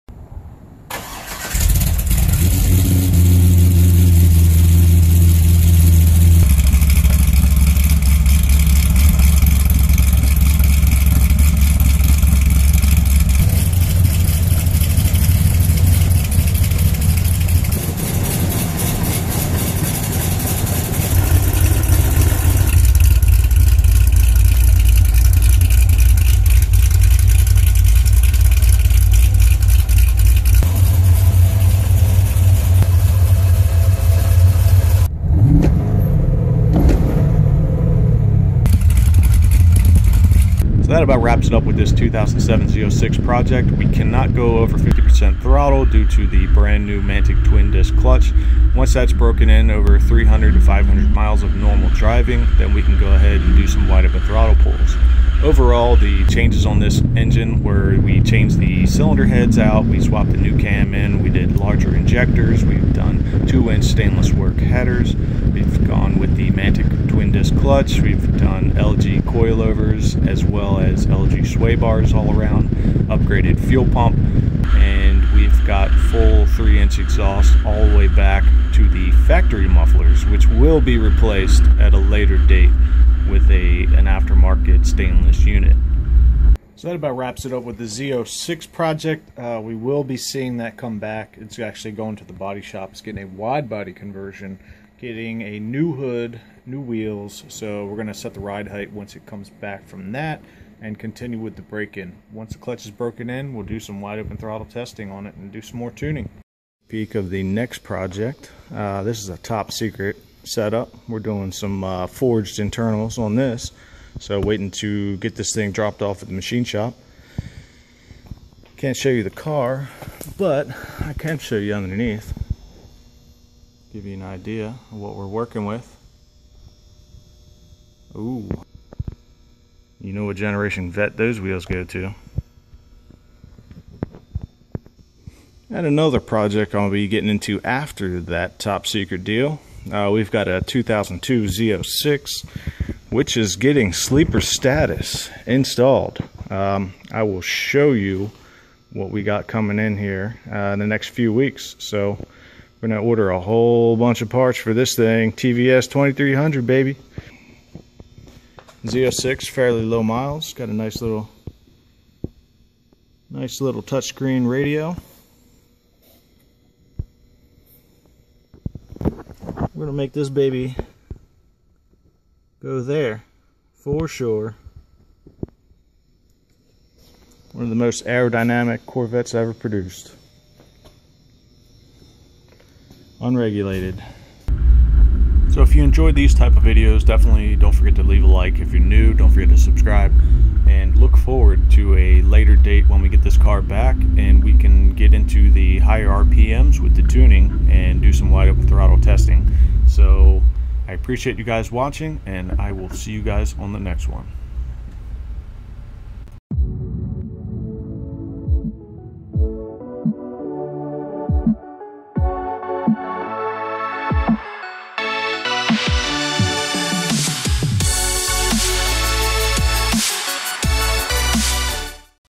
Billet Z Stage 1 Complete, Top C7, New projects announced.
Weve got tons of projects out here. The Billet Z06 is off to the body shop, and we have a new C7 LT1 to crank up the wick on. The Silver Sleeper Status C5Z06 plan has been laid out.